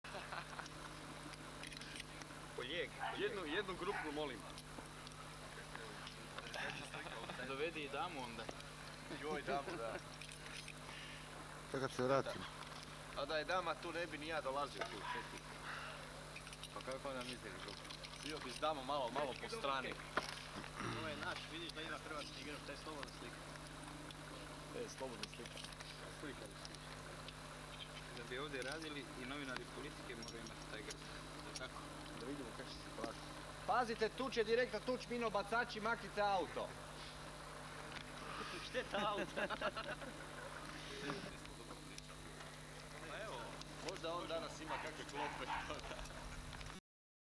HOS Vukovar 1991. prvi dio
Zagreb, vojarna Borongaj, druga polovica rujna 1991., neposredno prije pokreta za Vukovar. Dio ekipe na snimci Zdravko Špalj Papundek iz Senja, poginuo u Bogdanovcima, Tihomir Tomašić Tihica iz Velike Gorice, ubijen na Ovčari te Zvone Ćurković iz Zagreba, Josip Abel tada iz Velike Gorice, Viktorin Jurić Paša, tada iz Beča i ja, tada iz Velike Gorice.